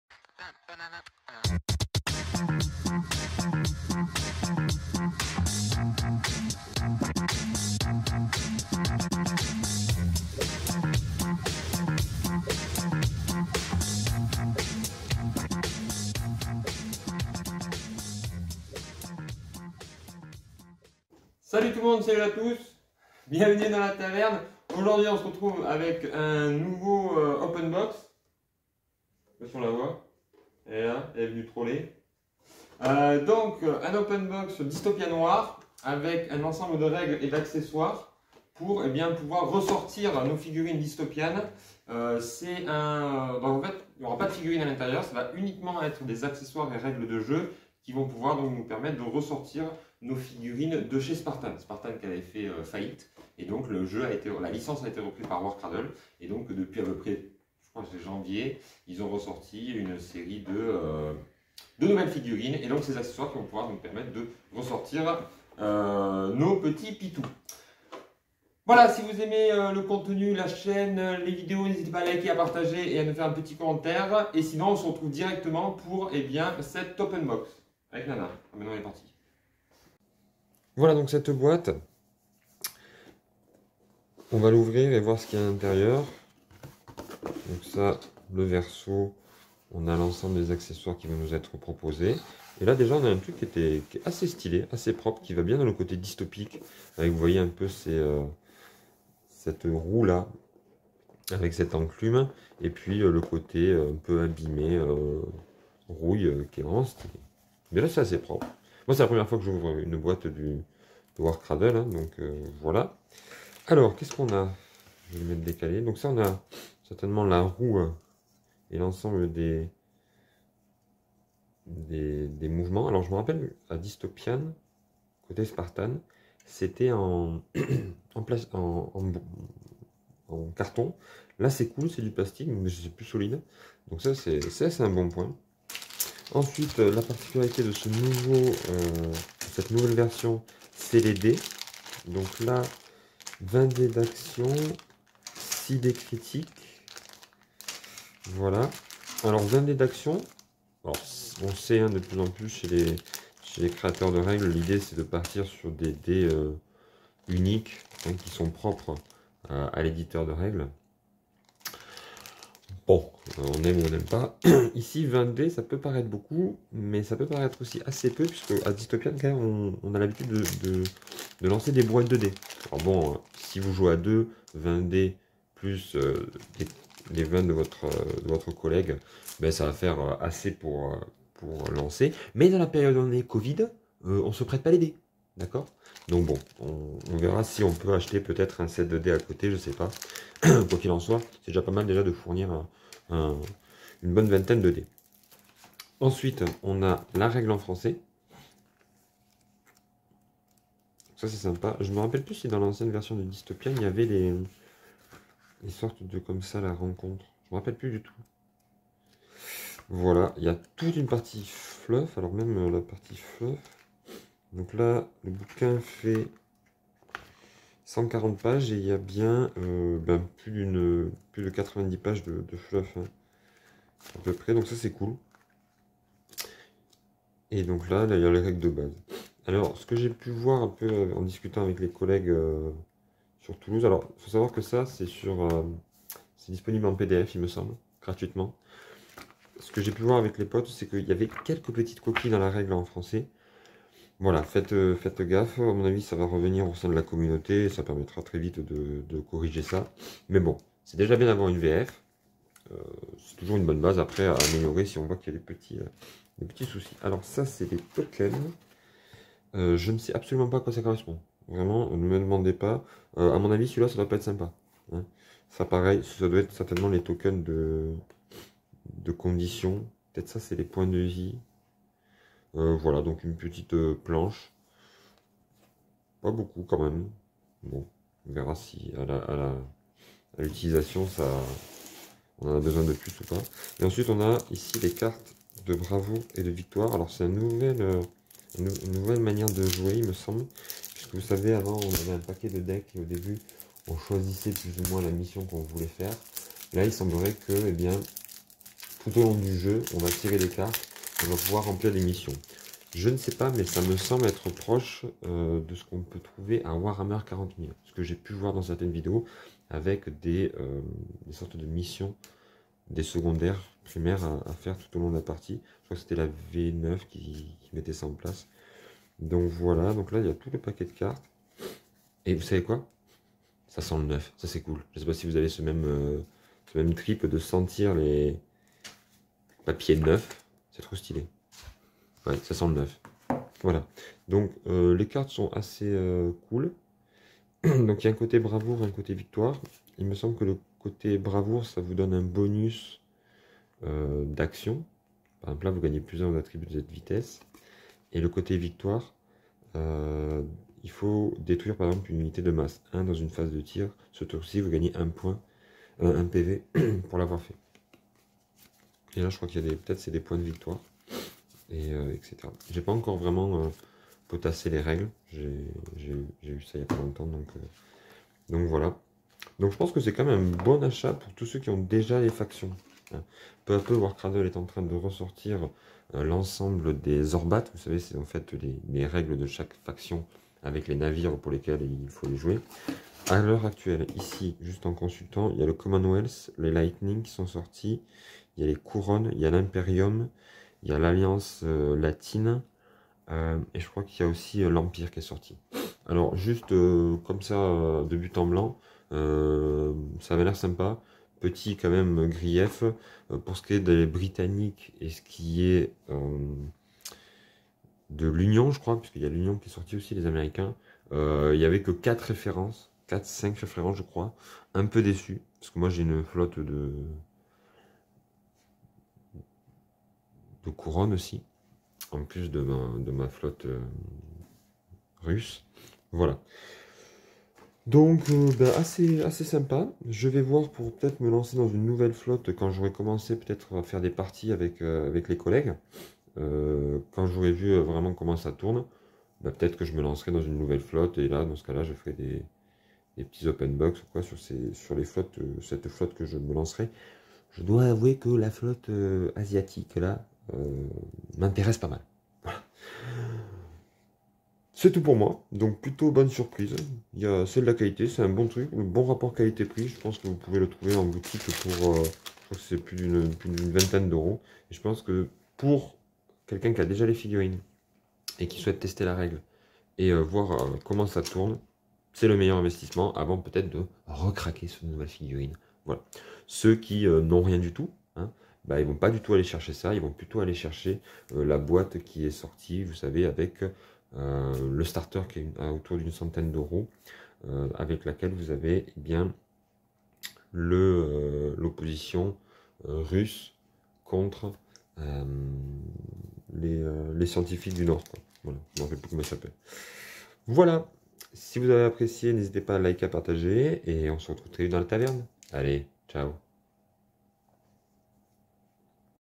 Open box Dystopian Wars boîte de règles
Salut tout le monde on se retrouve pour la découverte de la boîte de règle pour dystopian wars en VF.

Vous  retrouvez les produits dystopian sur  le site Maxi rêve vous pouvez leur dire que vous êtes un abonné de la chaine vous aurez 10% sur tout le site.

Et je ne touche aucune commission sur vos achats.